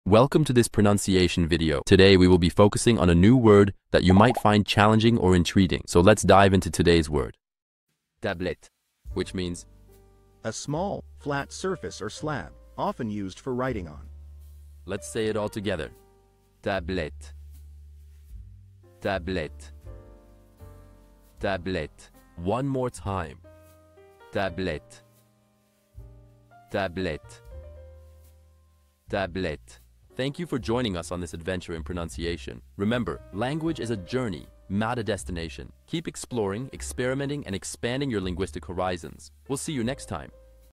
How to pronounce Tablette
Master the Pronunciation of 'Tablette Which means tablet' - which means : In Small, flat surface or slab, often used for writing on. 📝 with @BaguettePronunciation 🎓🗣️ - [Your Guide to French]

Learn to pronounce 'Tablette' perfectly in French with our easy and engaging tutorial by @BaguettePronunciation! 📖📽️ In this comprehensive video, we provide you with native speaker examples 🗣️👂 and phonetic breakdowns 🎵 to ensure you get the pronunciation just right.

How to say Tablet in French